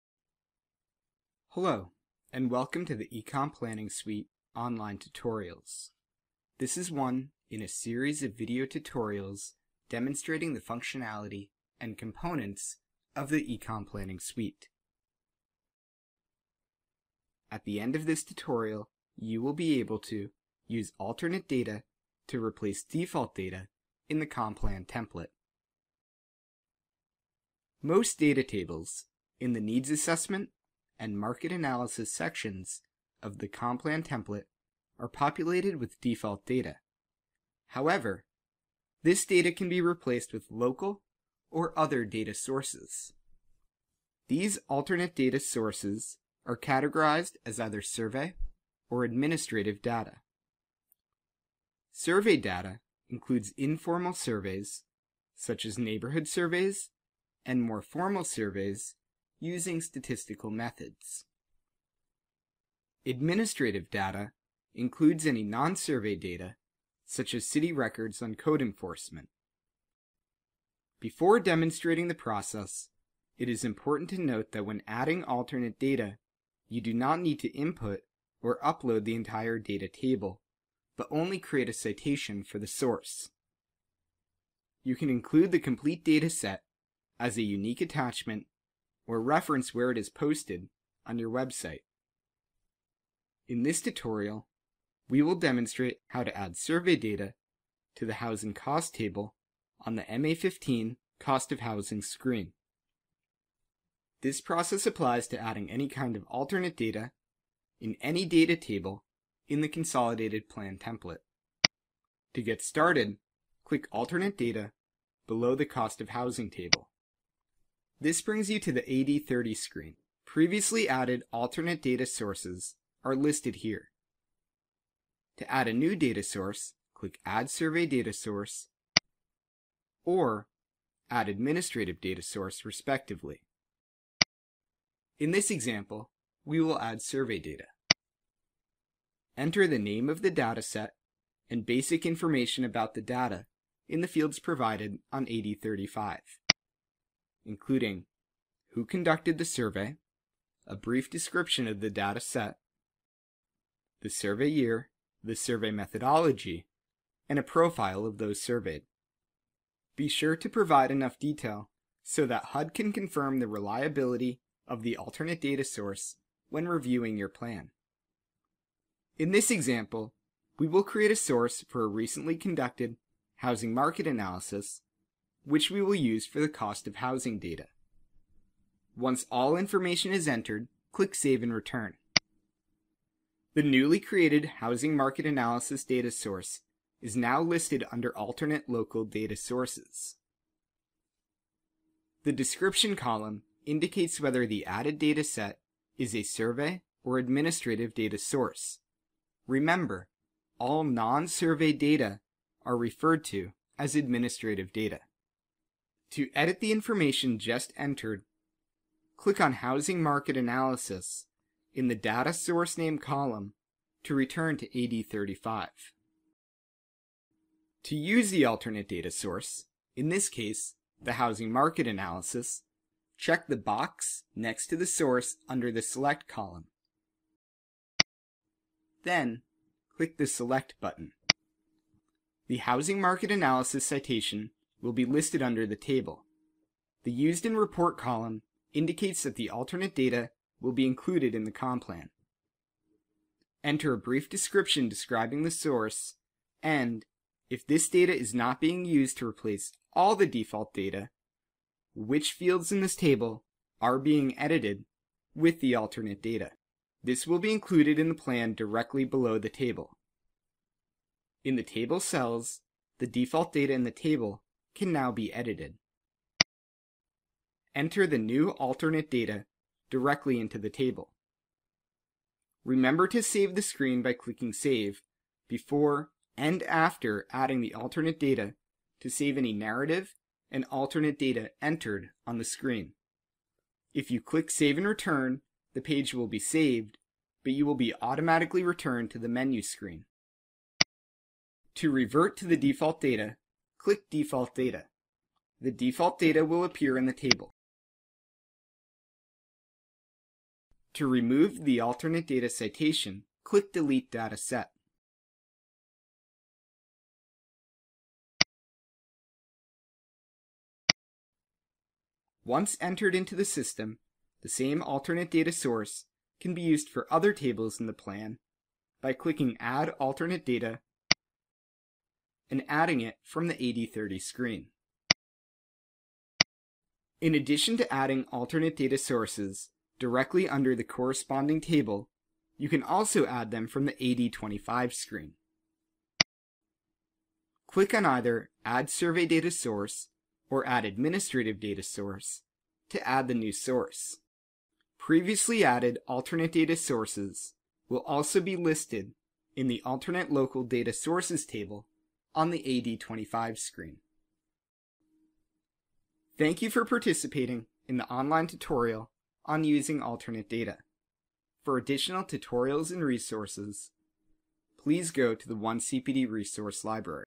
eCon Planning Suite e-Tutorial: Using Alternate Data in the Consolidated Plan Template
This e-tutorial is part of a series of e-tutorials demonstrating the functionality and components of the eCon Planning Suite. This video demonstrates how to replace default data in the Consolidated Plan template with local or alternate data sources.

To access related training materials & learn about getting credit via OneCPD Learn, visit OneCPD Training & Events